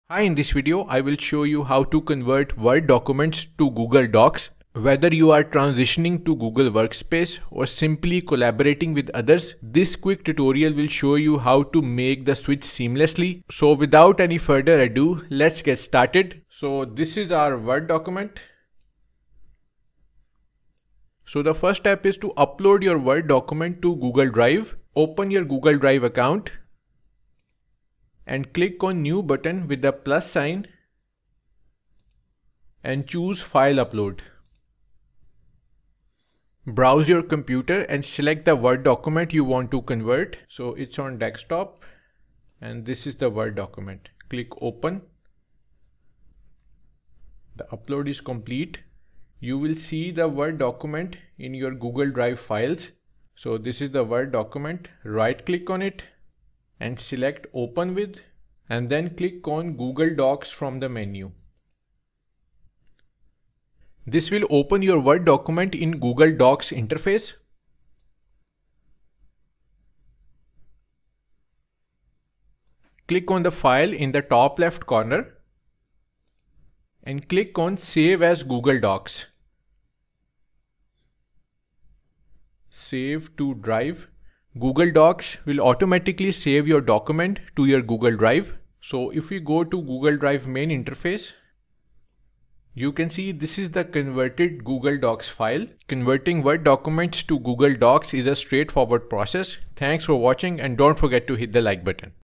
How to Convert Word Document to Google Docs
This video guides about how to convert word documents to google docs.

Converting Word documents to Google Docs is a straightforward process that allows you to take advantage of Google's collaborative features and cloud-based functionality.

Whether you're transitioning to Google Workspace or simply collaborating with others, this quick tutorial will show you how to make the switch seamlessly.

Here are the Steps to Convert Word to Google Docs:

1. Open Google Drive and make sure your are logged in to your account.

2. Click on the "New" button in the top left corner of the screen.

3. Select "File upload" from the dropdown menu.

4. A file explorer window will open. Navigate to the Word Document you want to convert.

5. Once upload is complete, right click on Word Document (.docx file) and select "Open with" and click on "Google Docs".

6. In Google Docs Interface, click on "File" and then choose "Save as Google Docs".

Google Docs will instantly save your newly converted document in your Google Drive.

I hope you know how to convert word document to google docs. If you're still having trouble, ask me about it in the comments, and I'll be sure to answer.